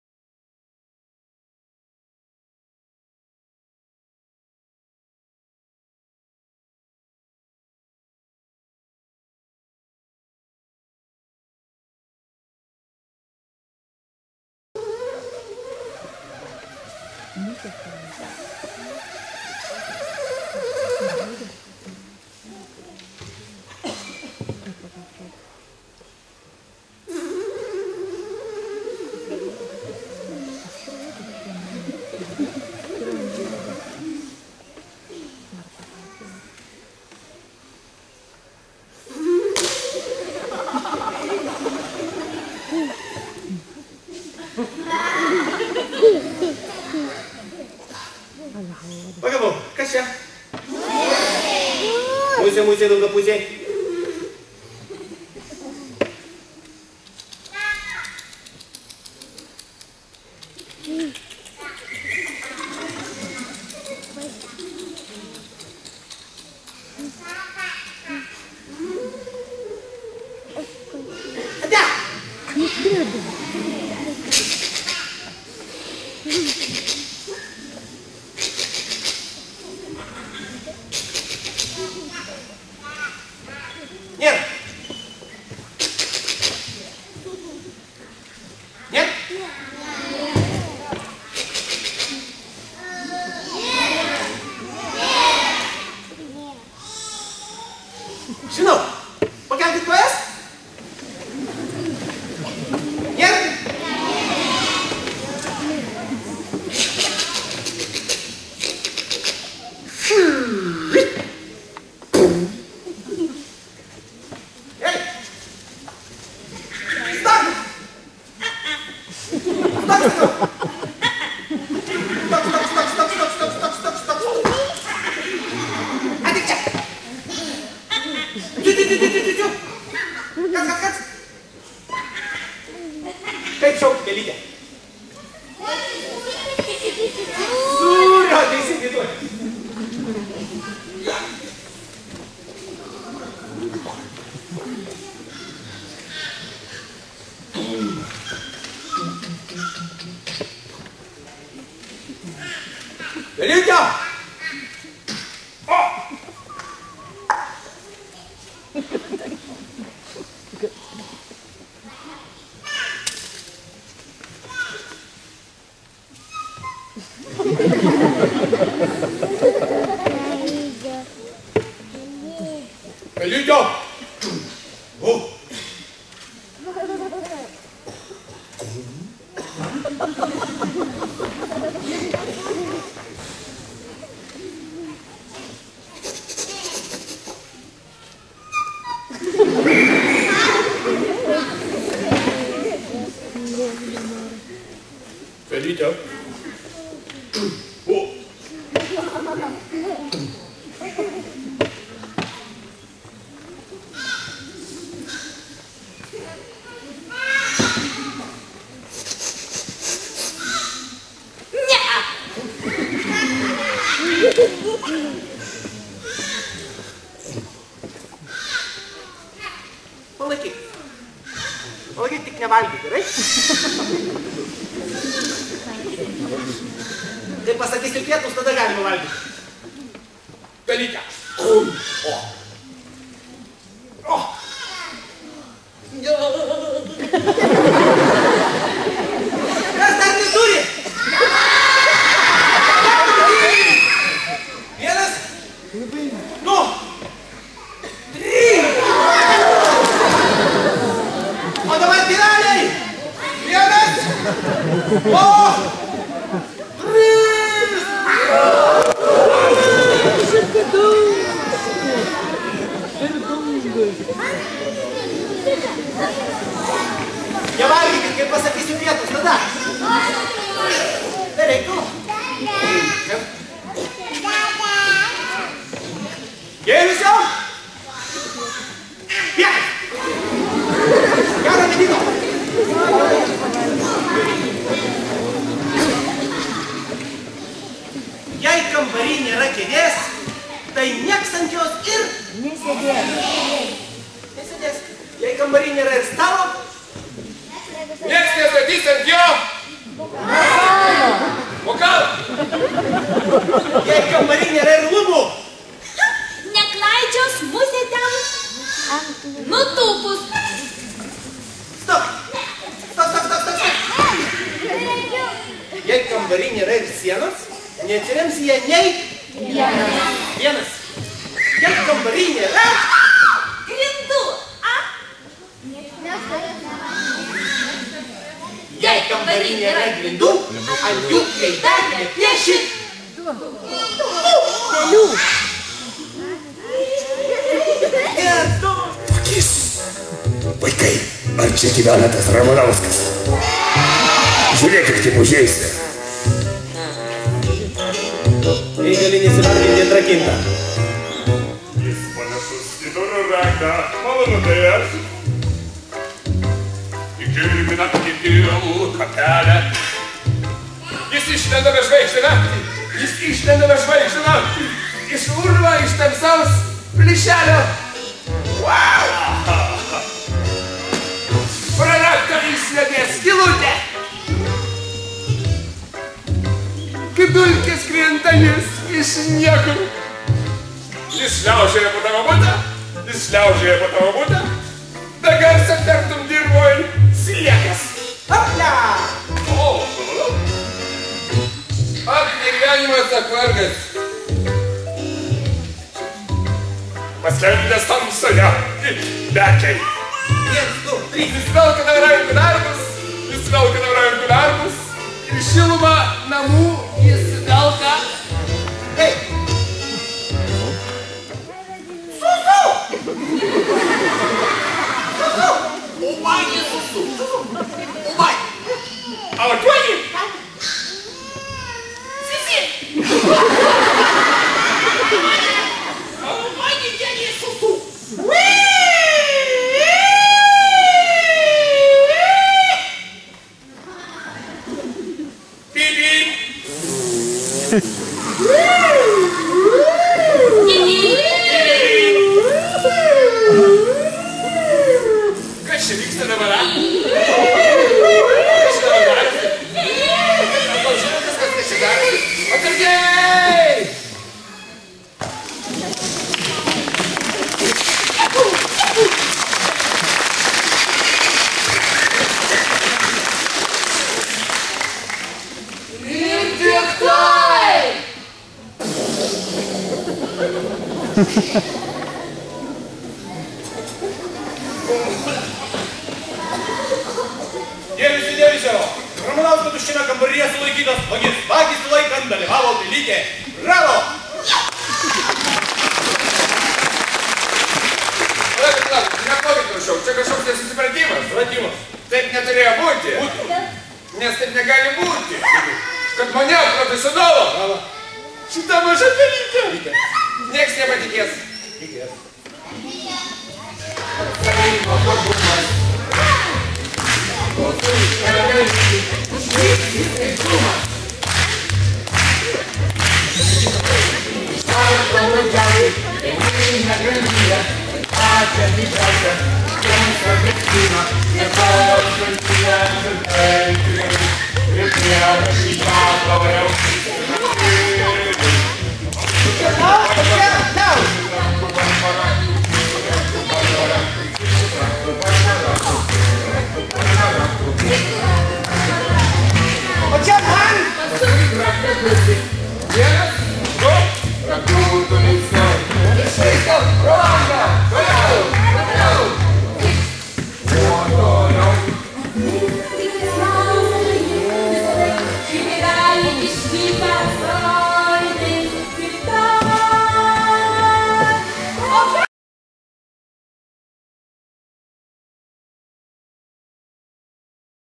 2007 m. Šeimų stovykla - "Teatriukas"
"Teatriuko" spektaklis "Kad vaikams būtų geriau".
2007 m. liepos 17 d., šeimų stovykloje Pakutuvėnuose.
Filmavo Vilma Drabulytė